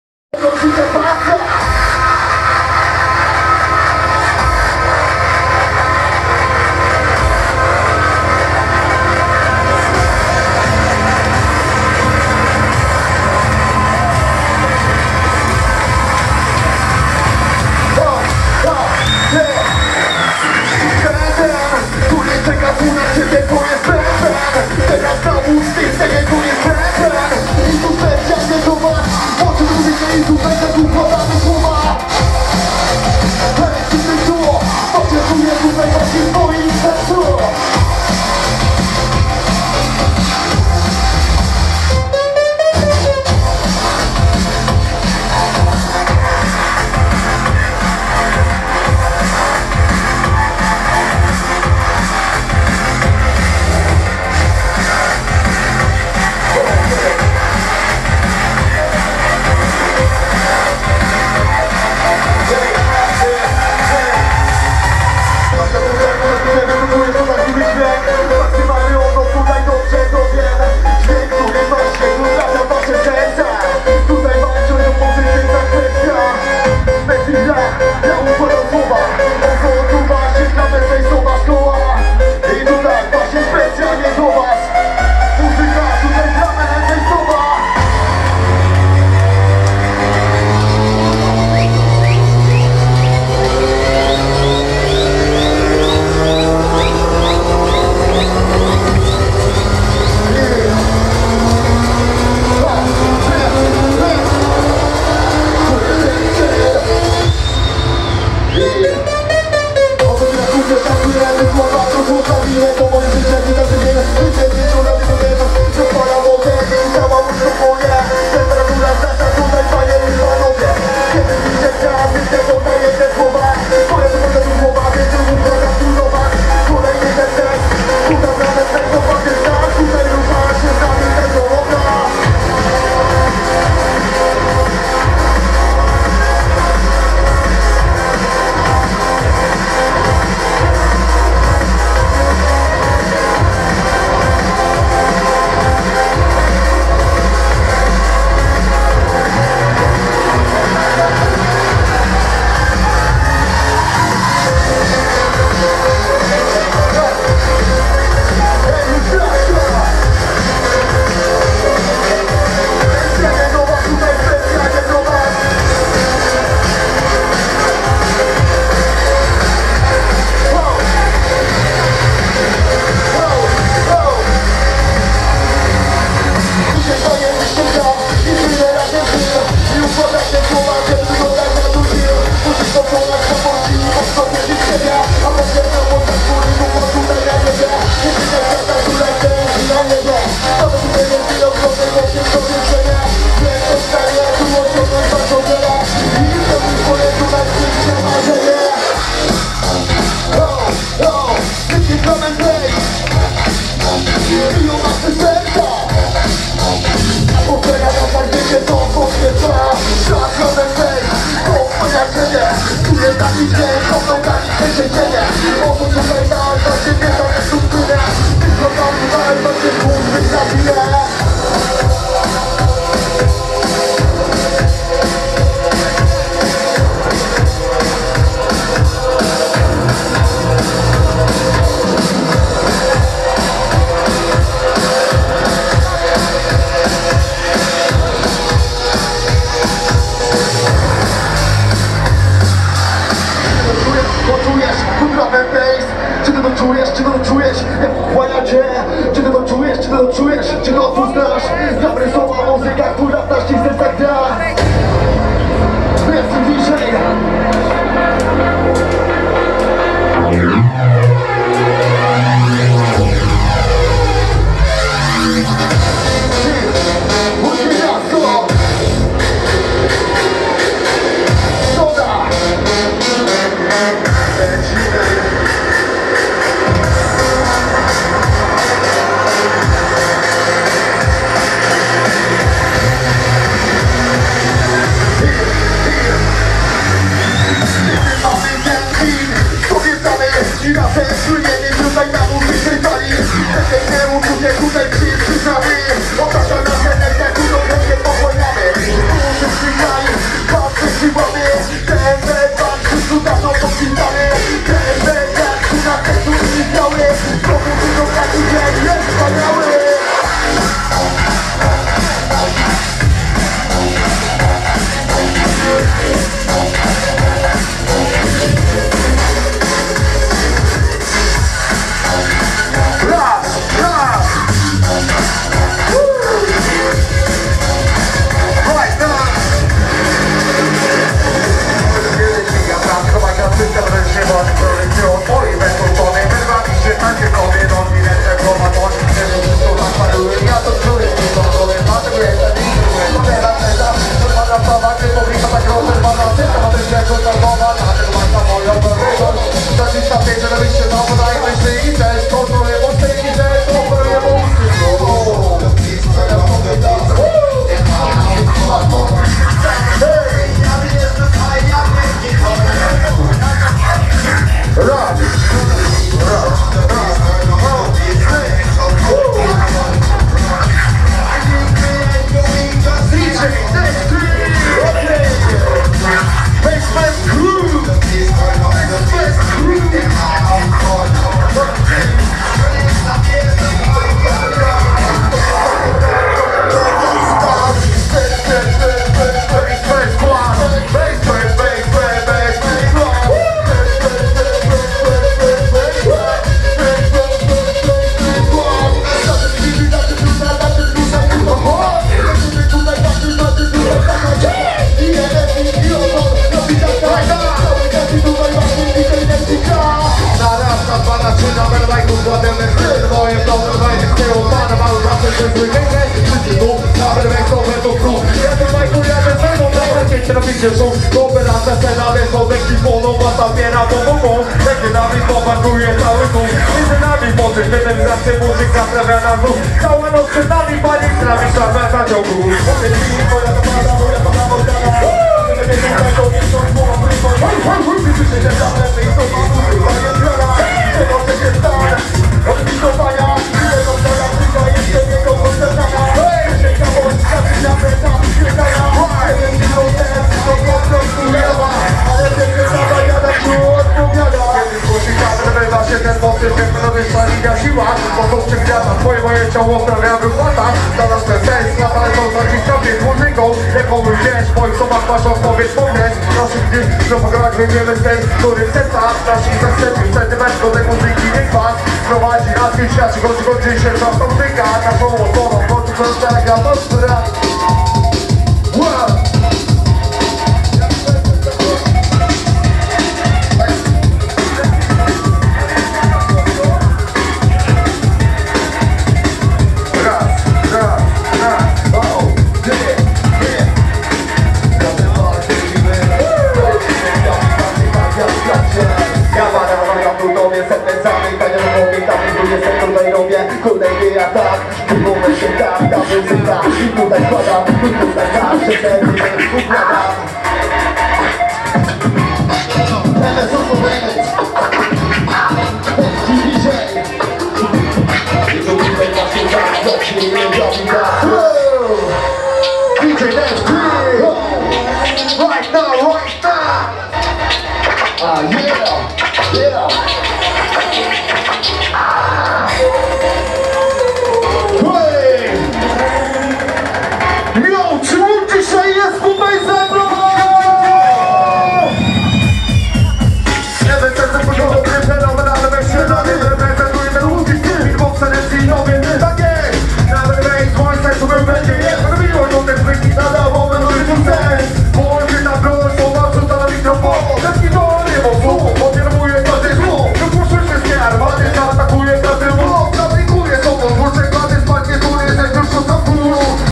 Nefti Mc Ozon Mc Emes Urodziny Akiry 05.09.2020 (part6)
Występ reprezentantów naszego Crew Dj Neftiego i Mc Ozona oraz Mc Emesa na imprezie urodzinowej Mc Akiry

Performance of the representatives of our Crew Dj Nefti and Mc Ozon and Mc Emes at the birthday party of Mc Akira